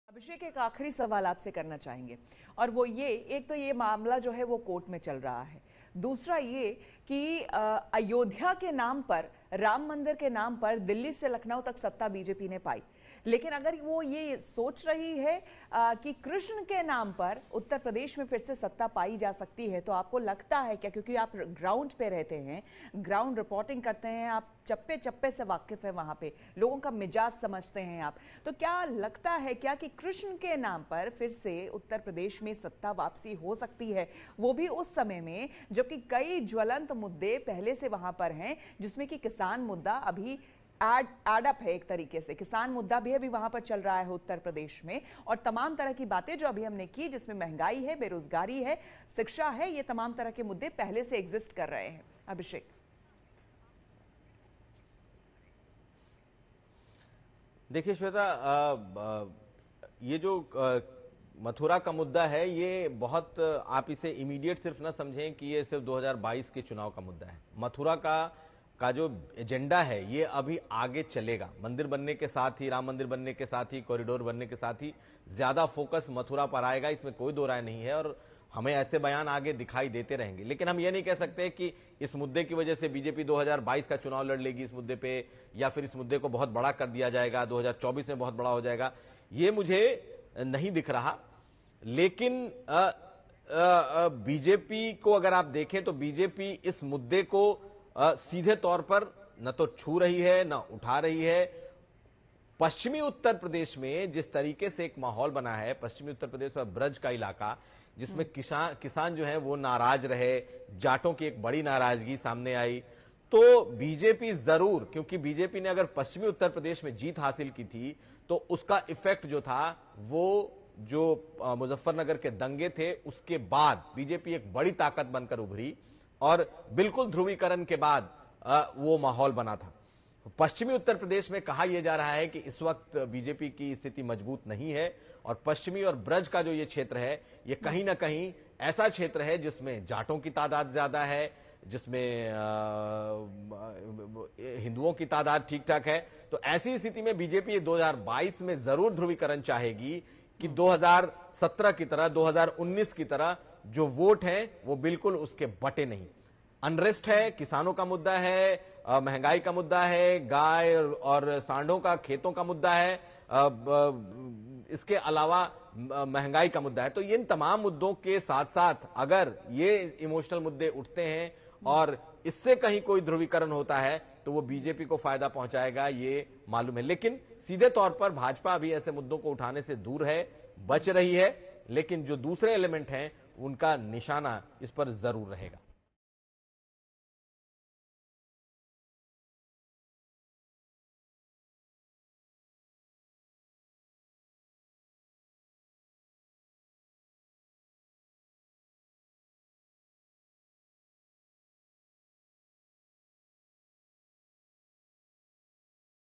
Aaj Ka Agenda: मथुरा का एजेंडा अभी आगे चलेगा, ये सिर्फ 2022 चुनाव के लिए नहीं ! Laatest News
UP Election 2022 में अब कुछ ही महीने बाकी है. लेकिन उससे पहले मथुरा में माहौल खराब नजर आ रहा है. मथुरा में धारा 144 लागू है. इसको लेकर आज तक के संवाददाता ने कहा कि चुनाव है इसलिए माहौल ऐसा दिख रहा है. उन्होंने कहा कि मथुरा का एजेंडा अभी आगे तक चलेगा.